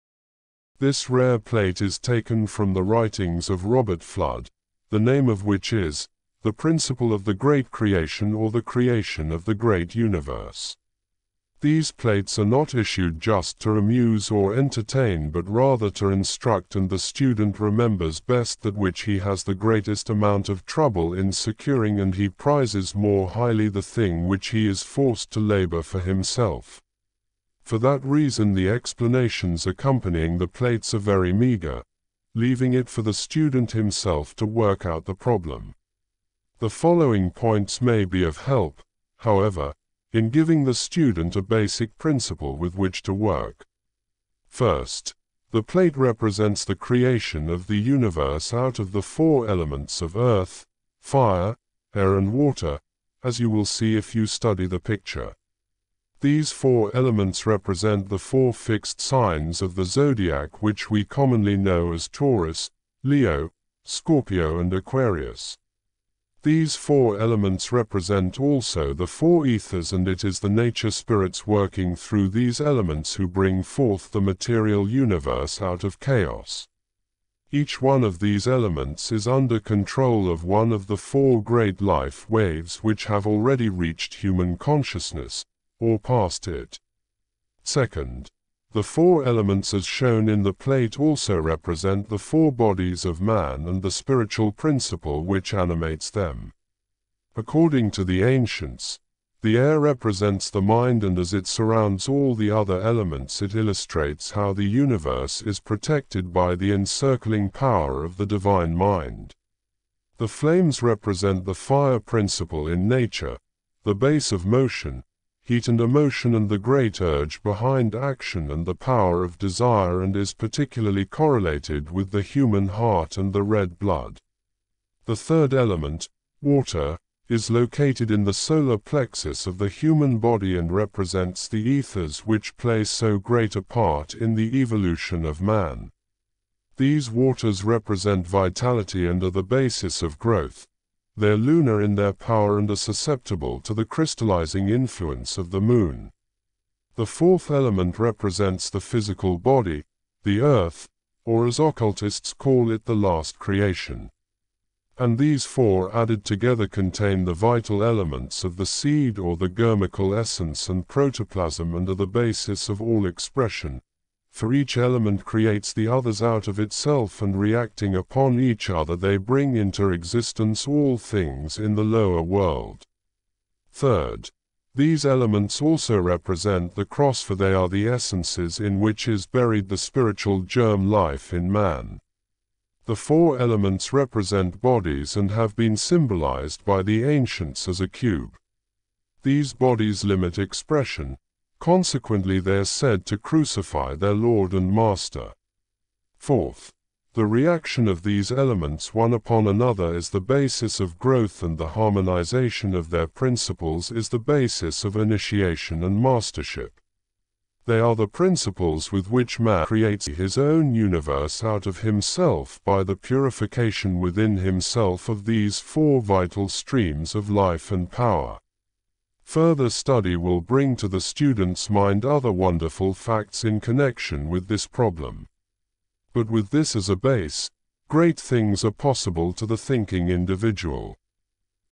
"De Macrocosmi Principiis" Rare Book Illustration with M.P.Hall's Commentary
Here is a short commentary on the symbolism of the plate taken from the writings of Robert Fludd which is called "De Macrocosmi Principiis" or "The Principle of the Great Creation" The commentary is  by M.P.Hall.